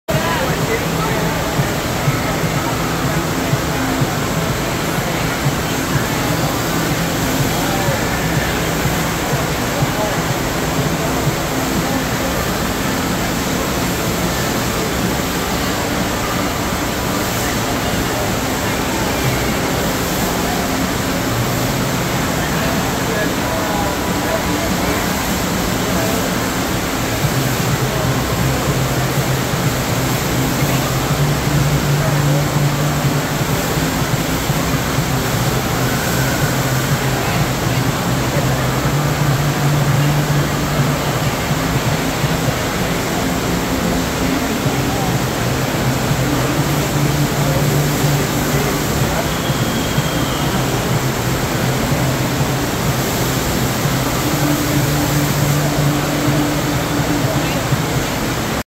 Practice Session at Epic Waters GP for the 2022 FlowRider World Championship(5)
Day 1 Practice during the US FlowRider Flow Tour World Championship at Epic Waters indoor waterpark Flowapalooza event 2022 in Grand Prairie, Texas.